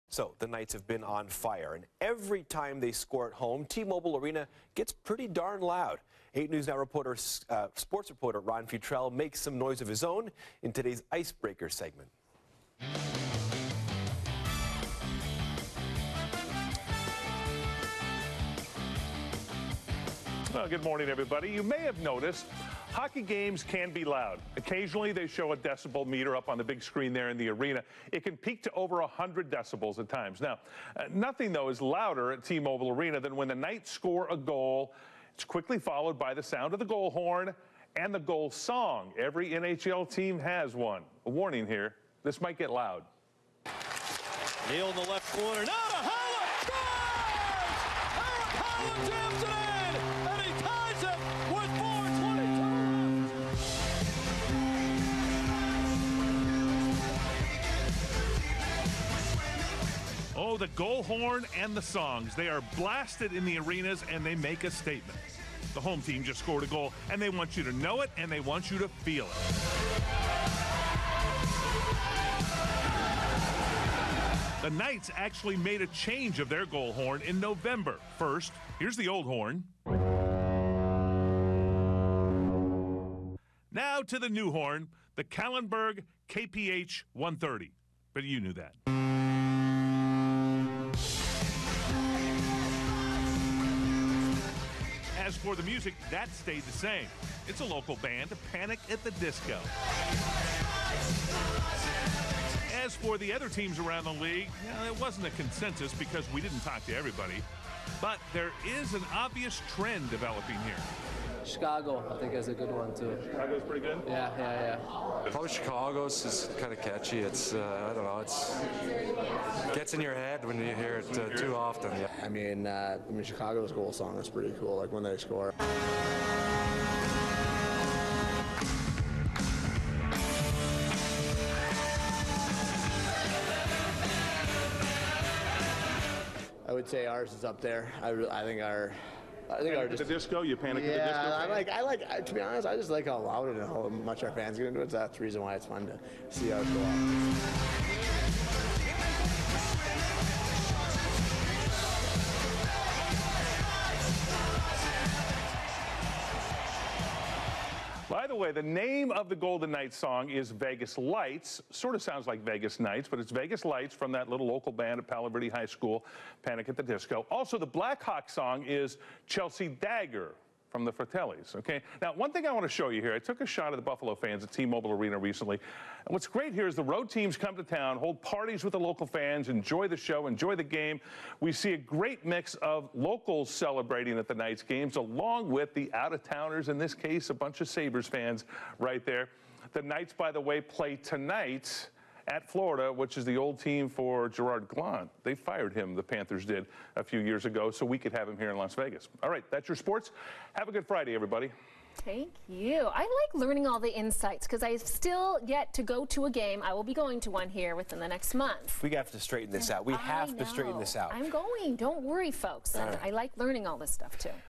Ron Futrell and Vegas Golden Knights Goal Horn and Song
Ron Futrell 8 News Now, Las Vegas reports on the goal horn and song for the Vegas Golden Knights NHL team. NHL GoalHorn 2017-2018 season.